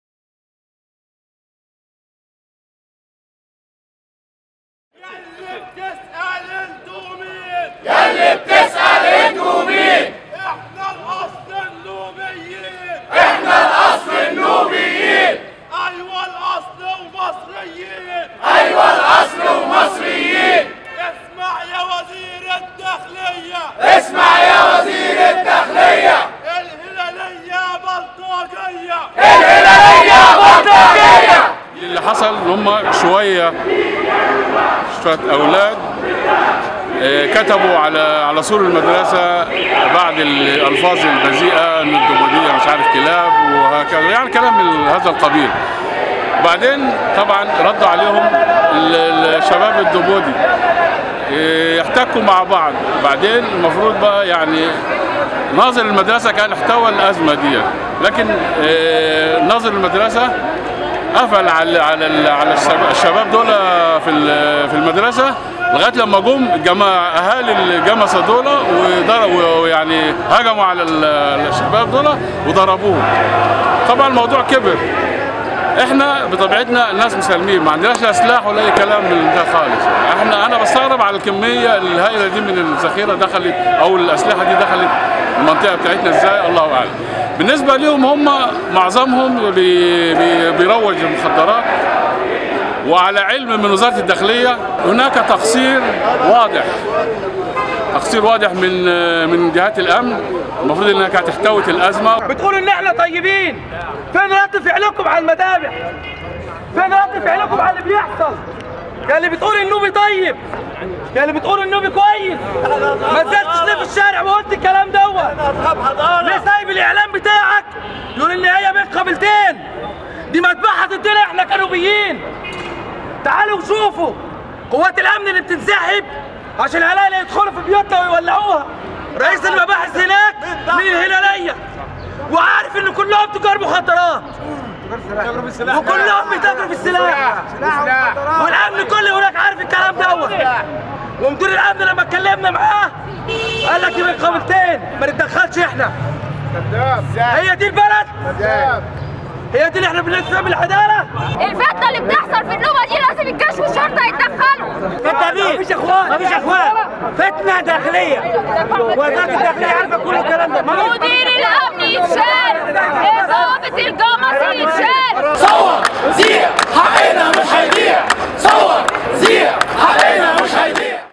وقفة النوبيين امام نقابة الصحفيين لوقف نزيف الدم بأسوان
تصوير محمود حسن ومحمد سالم  مونتاج زياد الغول